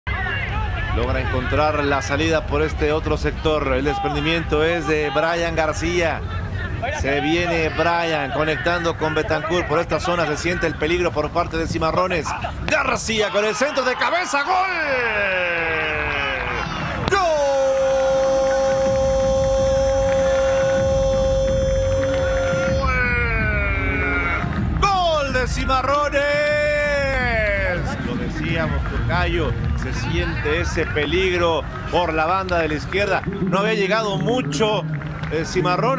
Gol de O. Villa | Cimarrones 2 - 0 Dorados | Jornada 7 - Guard1anes 2021
¡G⚽⚽⚽⚽L de Cimarrones! ⚽🔥

¡Llegó el segundo!

En la recta final del primer tiempo, Oscar Rai Villa saca este cabezazo en el área sinaloense y pone el 2-0 en el marcador.